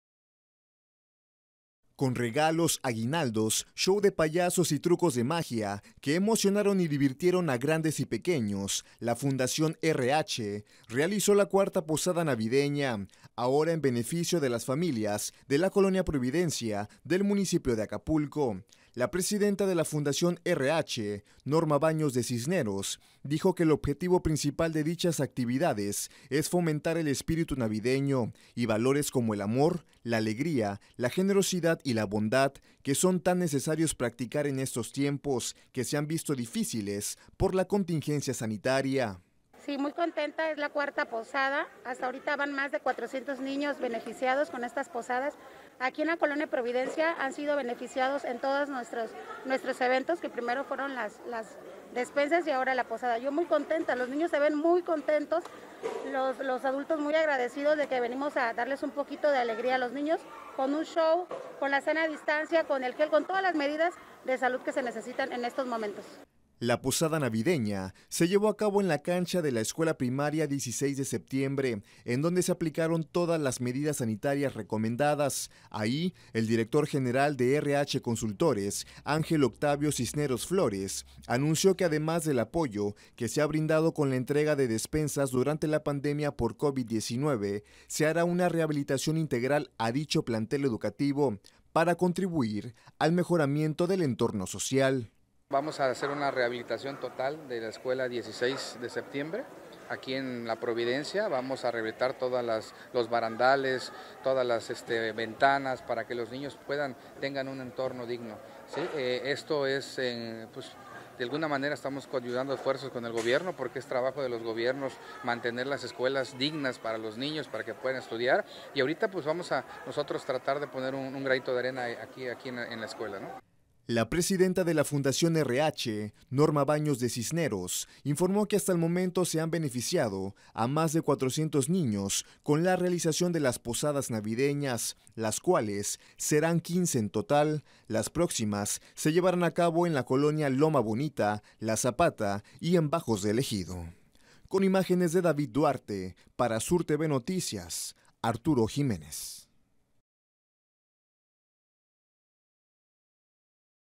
CUARTA POSADA NAVIDEÑA DE FUNDACIÓN RH EN LA PROVIDENCIA
Hasta el momento se han beneficiado a más de 400 niños con las posadas navideñas que realiza la Fundación Rh en diferentes colonias de Acapulco.